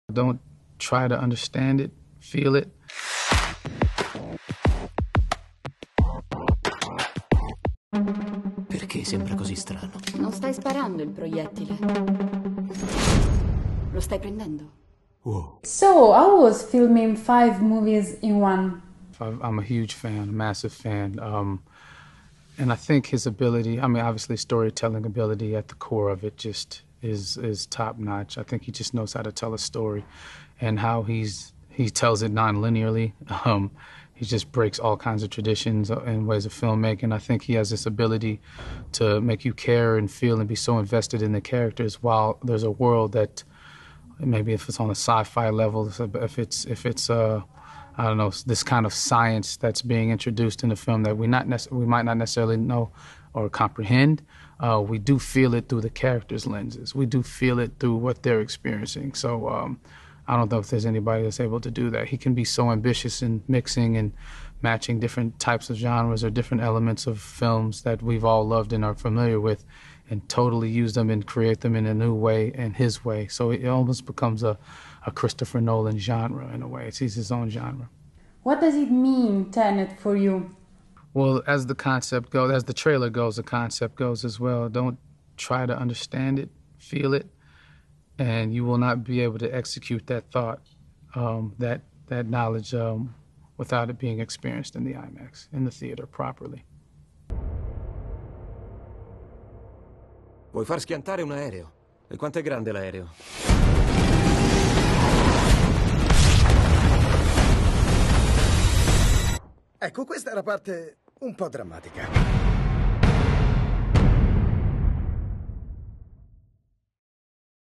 Tenet - La nostra intervista a John David Washington
La nostra Valentina Ariete ha intervistato il protagonista assoluto di Tenet, il nuovo, ambizioso film firmato Christopher Nolan.

Ecco cosa ci ha raccontato.